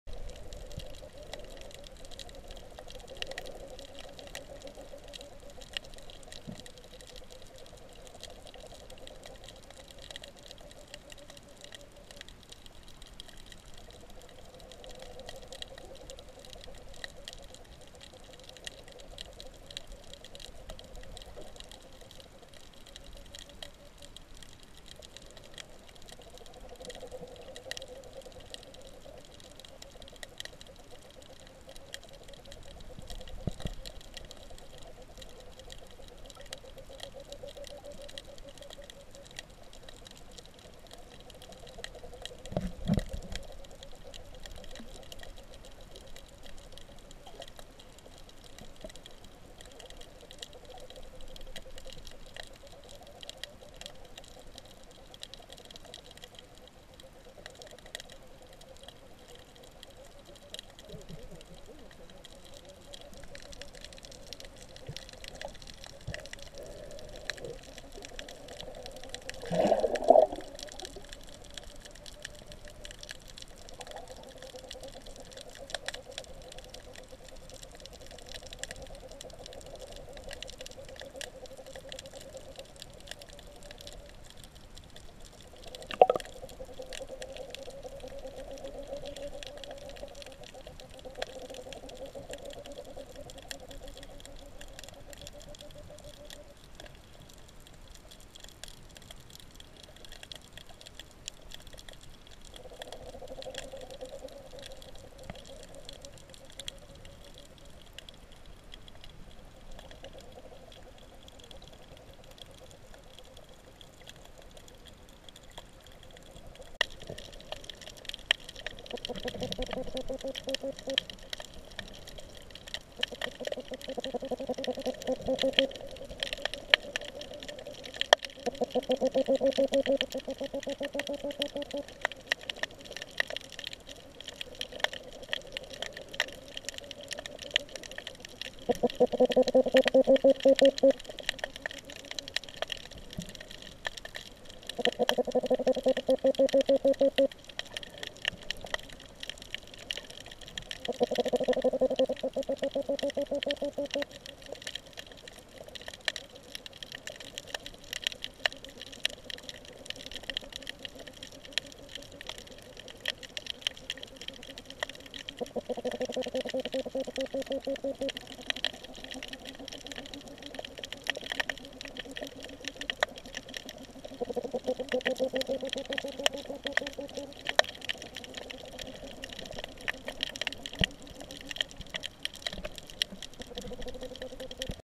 Raw snorkeling video from Baby Beach Aruba.
I didn't know the fish made noise under the water. Turn up the volume.